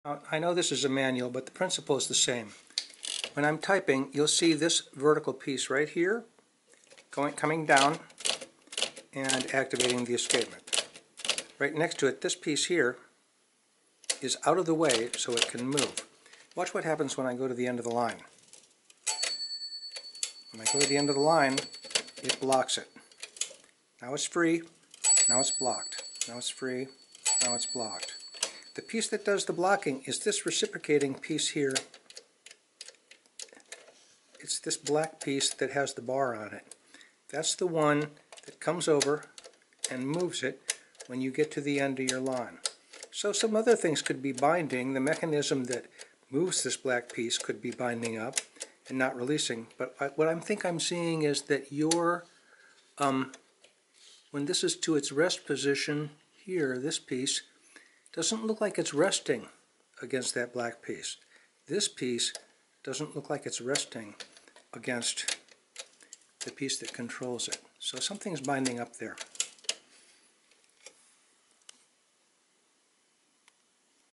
SCM typewriter escapement and margin lock/release
Trying to help out the owner of an early Smith-Corona electric portable whose escapement isn't working right -- we're zeroing in here on the part that swings in to lock the keys at the end of a line (and is supposed to release once you move the carriage back).